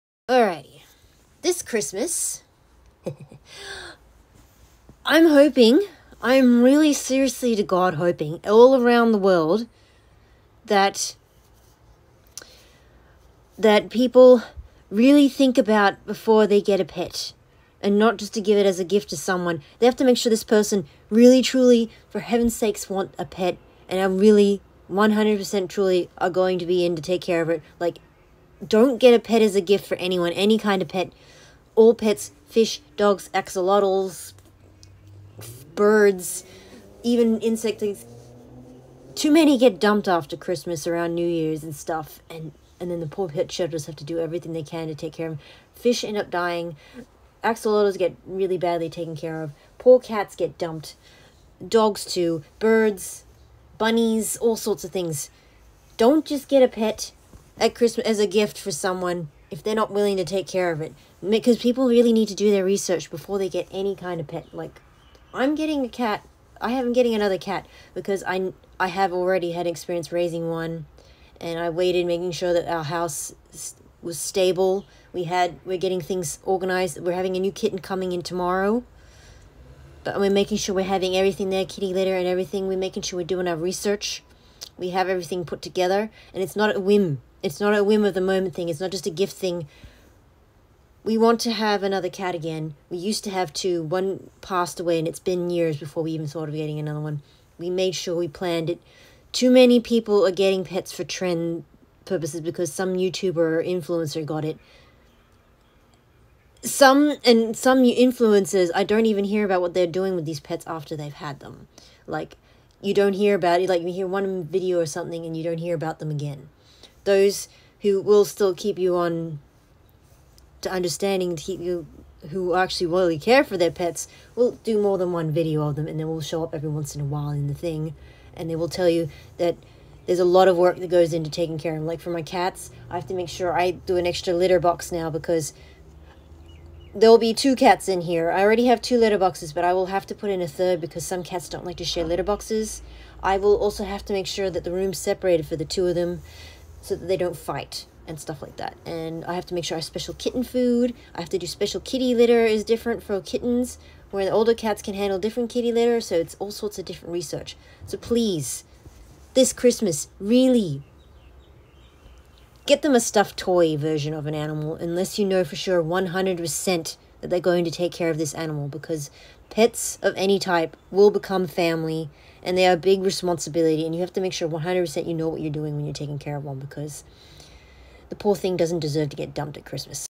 Pets are a lifelong commitment
Hello I’m Hobby Artist Alana & I run Alanaartdream on tumblr; YouTube; twitter; instagram and Tiktok

And too often I hear the awful sad news of the amount of pets dumped after being given as gifts to people who are unwilling to care for them or got bored of them

Pets are a long life commitment so you really need to do your research on how to care for them and what they need to live a long and happy life

Also do not get a pet just because it’s tending to have one

Make sure that you can and are willing to truly will to do right by your pet to love and care for the the best way that’s right for the both of you

Because no pet deserves to suffer from poor care & from being dumped